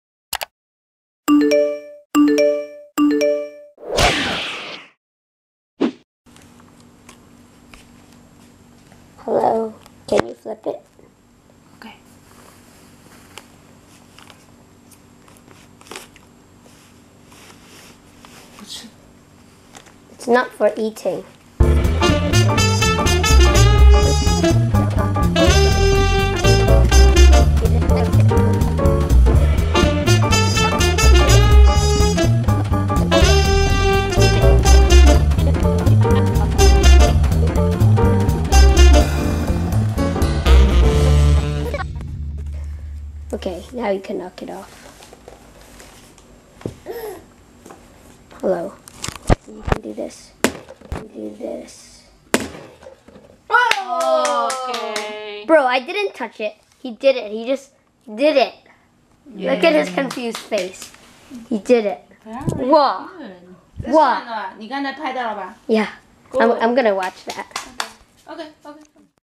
6 months old bottle flip!!
He is just 6 months old! And also did a bottle flip like my nephew which is 2 year old, also very epic:

No one will believe me in this: I came up with this idea for recording this, because before this flip, he did a CAP FLIP FIRST TRY! I know, sounds fake, but real. But it was against the wall, he actually didn't really flip it, but he dropped the bottle, then it bounced on the bottom, and flipped against the wall cap down! NO ONE WILL BELIEVE THIS!! (Other than me and my mom, because we saw it.)